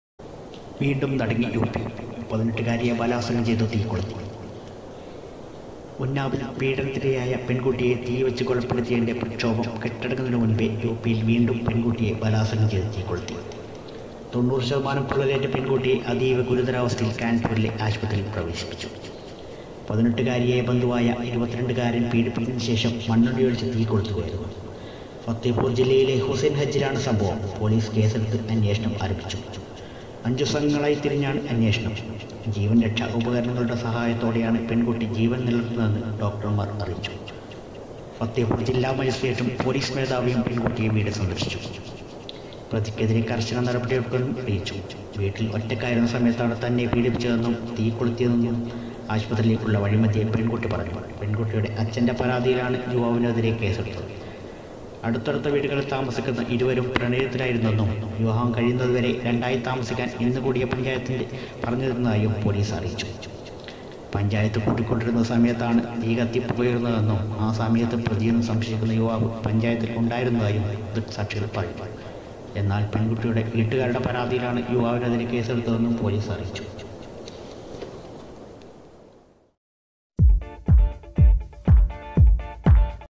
രാജ്യത്തെ നടുക്കി വീണ്ടും ക്രൂരത;യുപിയിൽ പെണ്‍കുട്ടിയെ ബലാത്സംഗംചെയ്ത് മണ്ണെണ്ണയൊഴിച്ച് തീകൊളുത്തി
രാജ്യത്തെ നടുക്കി വീണ്ടും ക്രൂരത; യുപിയിൽ പെണ്‍കുട്ടിയെ ബലാത്സംഗം ചെയ്ത ശേഷം മണ്ണെണ്ണയൊഴിച്ച് തീകൊളുത്തി, യു പി പീഡന പരമ്പര തുടരുന്നു, ആദിത്യ നാഥ്‌ നോക്കു കുത്തി